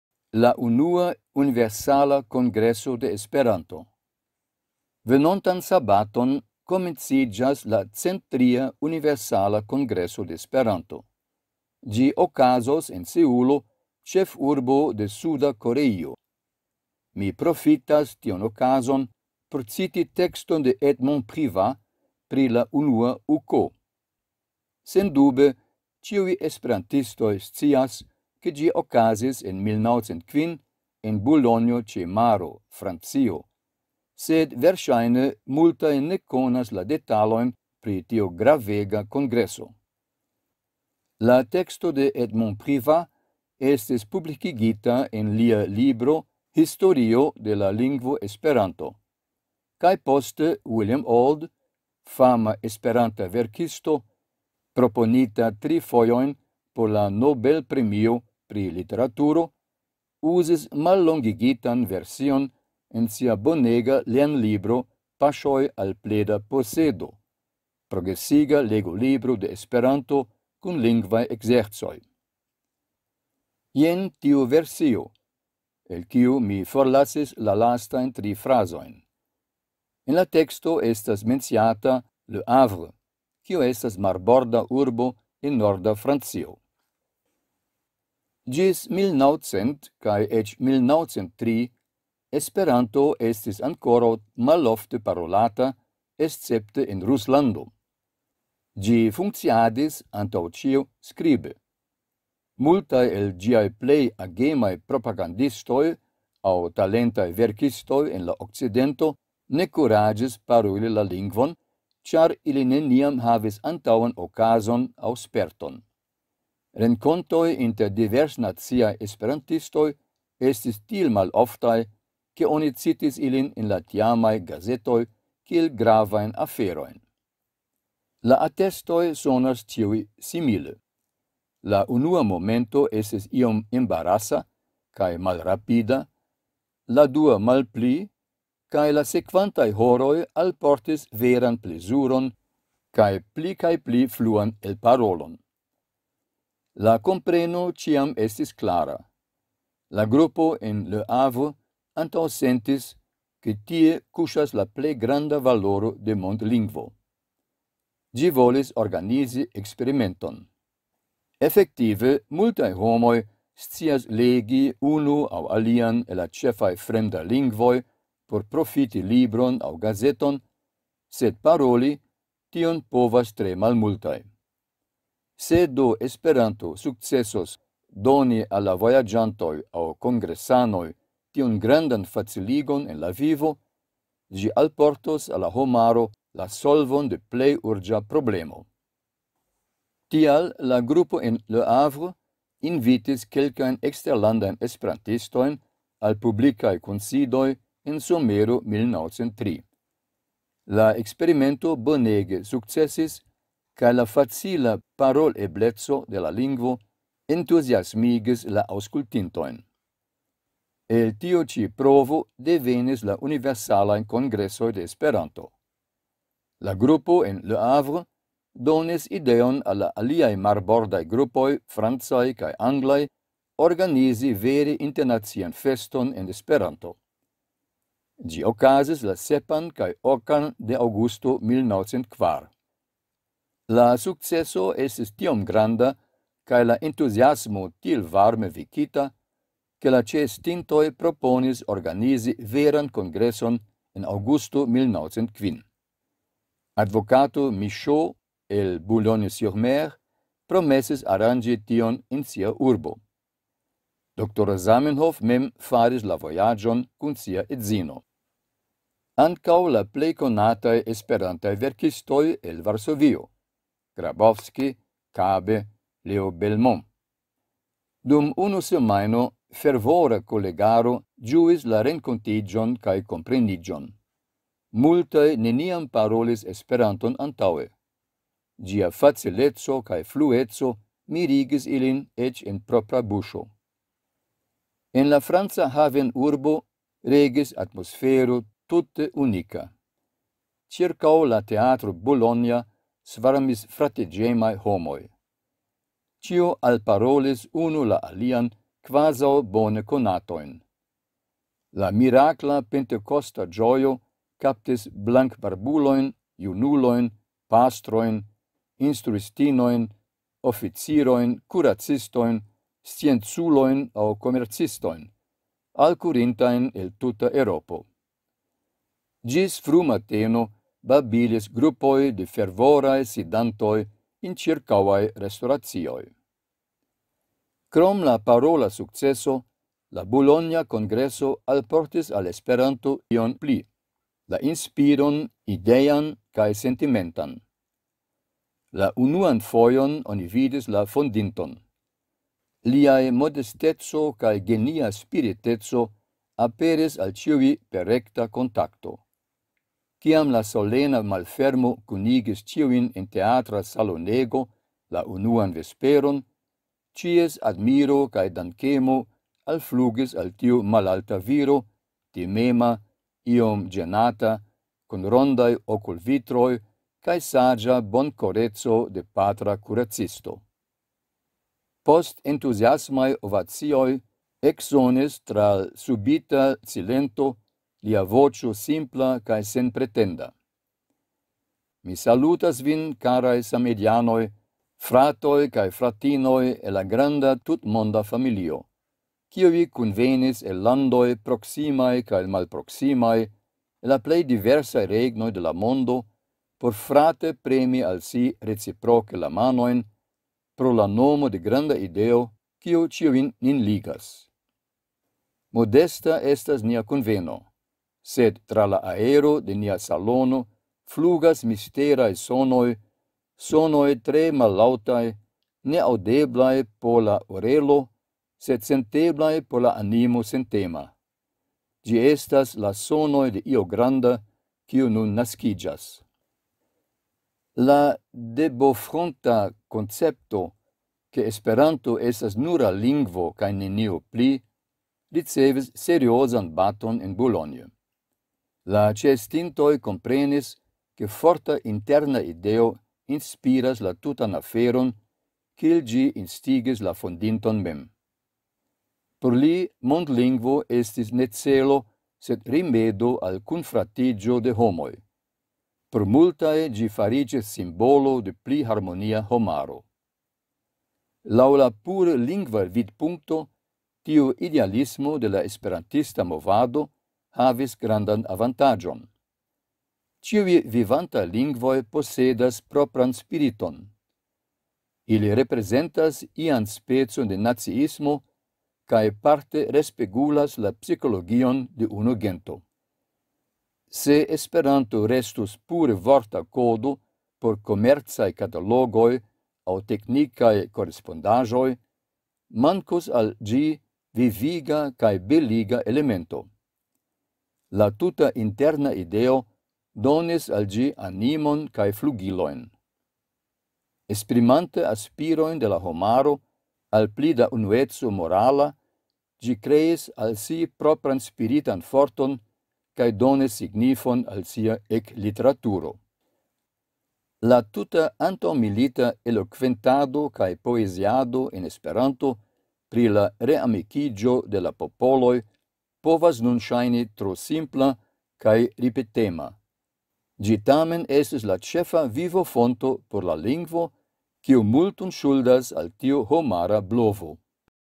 La Unua Universala Kongreso de Esperanto
Ĉi tiu artikolo aperis la 18-an de julio 2017 en la Esperanta Retradio Vidu la tekston

Venontan sabaton komenciĝas la 103-a Universala Kongreso de Esperanto. Ĝi okazos en Seulo, ĉefurbo de Suda Koreio. Mi profitas tiun okazon por citi tekston de Edmond Privat pri la unua UK. Sendube ĉiuj esperantistoj scias, ke ĝi okazis en 1905 en Bulonjo-ĉe-Maro, Francio, sed verŝajne multaj ne konas la detalojn pri tiu gravega kongreso.
La teksto de Edmond Privat estis publikigita en lia libro “Historio de la lingvo Esperanto”, kaj poste William Auld – fama esperanta verkisto, proponita tri fojojn por la Nobelpremio pri Literaturo – uzis mallongigitan version en sia bonega lernlibro “Paŝoj al Plena Posedo – Progresiga Legolibro de Esperanto kun lingvaj ekzercoj”.
Jen tiu versio (el kiu mi forlasis la lastajn tri frazojn; en la teksto estas menciata “Le Havre”, kiu estas marborda urbo en norda Francio):
“Ĝis 1900 kaj eĉ 1903 Esperanto estis ankoraŭ malofte parolata escepte en Ruslando. Ĝi funkciadis antaŭ ĉio skribe. Multaj el ĝiaj plej agemaj propagandistoj aŭ talentaj verkistoj en la okcidento ne kuraĝis paroli la lingvon, ĉar ili neniam havis antaŭan okazon aŭ sperton. Renkontoj inter diversnaciaj esperantistoj estis tiel maloftaj, ke oni citis ilin en la tiamaj gazetoj kiel gravajn aferojn.
La atestoj sonas ĉiuj simile. La unua momento estis iom embarasa kaj malrapida, la dua malpli, kaj la sekvantaj horoj alportis veran plezuron kaj pli kaj pli fluan elparolon. La kompreno ĉiam estis klara. La grupo en Le Hâvre antaŭsentis, ke tie kuŝas la plej granda valoro de mondlingvo. Ĝi volis organizi eksperimenton. Efektive multaj homoj scias legi unu aŭ alian el la ĉefaj fremdaj lingvoj por profiti libron aŭ gazeton, sed paroli, tion povas tre malmultaj. Se do Esperanto sukcesos doni al la vojaĝantoj aŭ kongresanoj tiun grandan faciligon en la vivo, ĝi alportos al la homaro la solvon de plej urĝa problemo.
Tial la grupo en Le Hâvre invitis kelkajn eksterlandajn esperantistojn al publikaj kunsidoj en somero 1903. La eksperimento bonege sukcesis kaj la facila parolebleco de la lingvo entuziasmigis la aŭskultintojn.
El tiu ĉi provo devenis la Universalaj Kongresoj de Esperanto. La grupo en Le Hâvre donis ideon al la aliaj marbordaj grupoj francaj kaj anglaj organizi vere internacian feston en Esperanto. Ĝi okazis la 7-an kaj 8-an de Aŭgusto 1904. La sukceso estis tiom granda kaj la entuziasmo tiel varme vekita, ke la ĉeestintoj proponis organizi veran kongreson en Aŭgusto 1905. Advokato Michaux el Boulogne sur Mer promesis aranĝi tion en sia urbo.
D-ro Zamenhof mem faris la vojaĝon kun sia edzino. Ankaŭ la plej konataj esperantaj verkistoj el Varsovio: Grabowski, Kabe, Leo Belmont. Dum unu semajno fervora kolegaro ĝuis la renkontiĝon kaj kompreniĝon. Multaj neniam parolis Esperanton antaŭe. Ĝia facileco kaj flueco mirigis ilin eĉ en propra buŝo.
En la franca havenurbo regis atmosfero tute unika. Ĉirkaŭ la teatro Bulonja svarmis fratiĝemaj homoj. Ĉiu alparolis unu la alian kvazaŭ bone konatojn. La mirakla pentekosta ĝojo kaptis blankbarbulojn, junulojn, pastrojn, instruistinojn, oficirojn, kuracistojn, scienculojn aŭ komercistojn, alkurintajn el tuta Eŭropo. Ĝis frumateno babilis grupoj de fervoraj sidantoj en ĉirkaŭaj restoracioj.
Krom la parola sukceso, la Bulonja kongreso alportis al Esperanto ion pli: la inspiron idean kaj sentimentan. La unuan fojon oni vidis la fondinton. Liaj modesteco kaj genia spiriteco aperis al ĉiuj per rekta kontakto. Kiam la solena malfermo kunigis ĉiujn en teatra salonego la unuan vesperon, ĉies admiro kaj dankemo alflugis al tiu malalta viro, timema, iom ĝenata, kun rondaj okulvitroj kaj saĝa bonkoreco de patra kuracisto.
Post entuziasmaj ovacioj eksonis tra l'subita silento lia voĉo simpla kaj senpretenda: ‘Mi salutas vin, karaj samideanoj, fratoj kaj fratinoj el la granda tutmonda familio, kiuj kunvenis el landoj proksimaj kaj malproksimaj, el la plej diversaj regnoj de la mondo por frate premi al si reciproke la manojn, pro la nomo de granda ideo, kiu ĉiujn nin ligas ... Modesta estas nia kunveno .. , sed tra la aero de nia salono flugas misteraj sonoj, sonoj tre mallaŭtaj, ne aŭdeblaj por la orelo, sed senteblaj por la animo sentema: ĝi estas la sonoj de io granda, kiu nun naskiĝas ... .’
La de Beaufront-a koncepto, ke "Esperanto estas nura lingvo kaj nenio pli”  ricevis seriozan baton en Boulogne. La ĉeestintoj komprenis, ke forta "interna ideo” inspiras la tutan aferon kiel ĝi instigis la fondinton mem. Por li mondlingvo estis ne celo, sed rimedo al kunfratiĝo de homoj. Por multaj ĝi fariĝis simbolo de pli harmonia homaro.
Laŭ la pure lingva vidpunkto, tiu idealismo de la esperantista movado havis grandan avantaĝon. Ĉiuj vivantaj lingvoj posedas propran "spiriton". Ili reprezentas ian specon de naciismo kaj parte respegulas la psikologion de unu gento. Se Esperanto restus pure vorta kodo por komercaj katalogoj aŭ teknikaj korespondaĵoj, mankus "